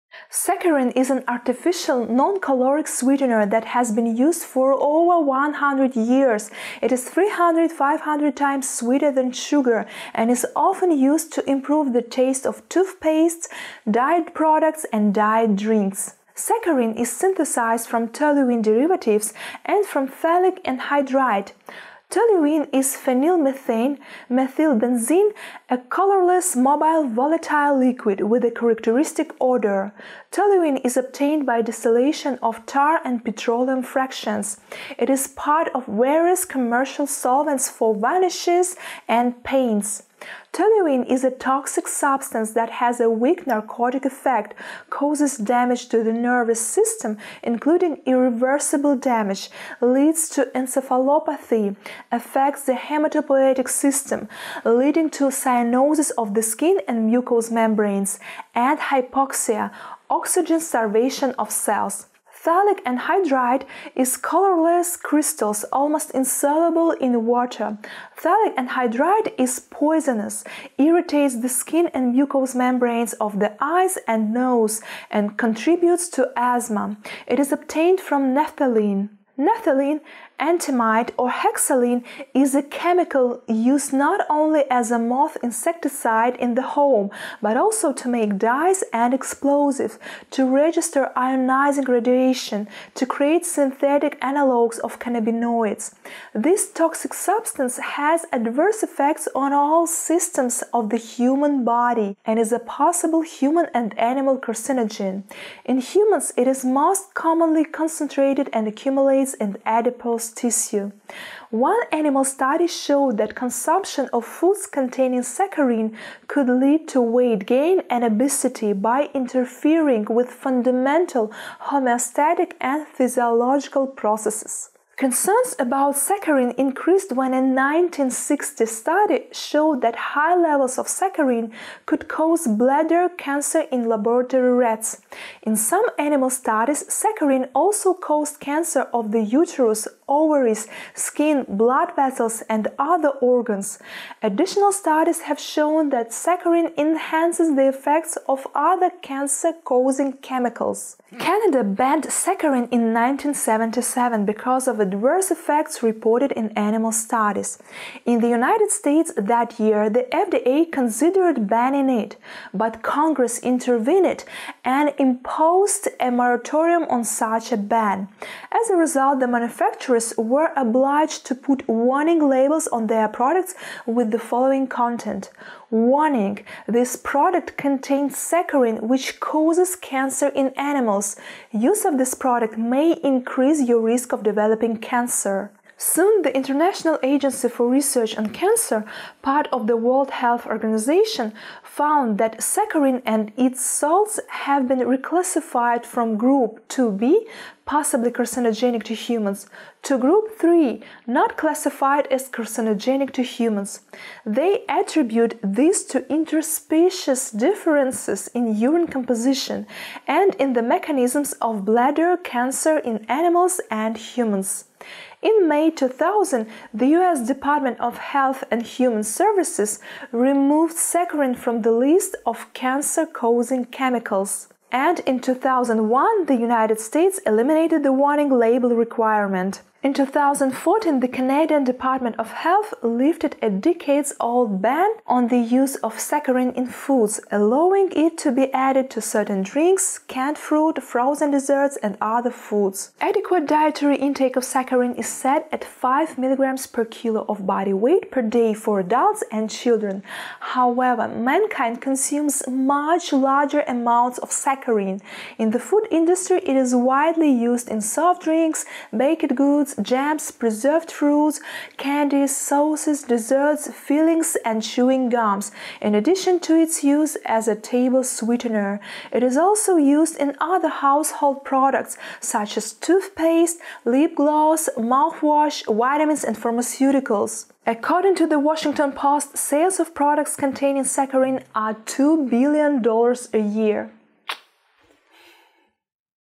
Is Saccharin a toxic Carcinogen?
Over 40 years, saccharin has gone from being a banned carcinogenic substance to a popular and “safe” sweetener. How could this happen? Let's trace the path of saccharin.
TIME STAMPS:
00:16 What saccharin is made of
02:05 Does saccharin cause cancer?
03:05 Doesn't saccharin cause cancer?
REFERENCES
Sugar Substitutes Linked to Weight Gain. Tracy Hampton, PhD. JAMA. 2008;299(18):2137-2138. doi:10.1001/jama.299.18.2137.
The Washington Post. 18-Month Saccharin Sale Bill, With Cancer Warning, Is Voted
Center for Science in the Public Interest. Saccharin.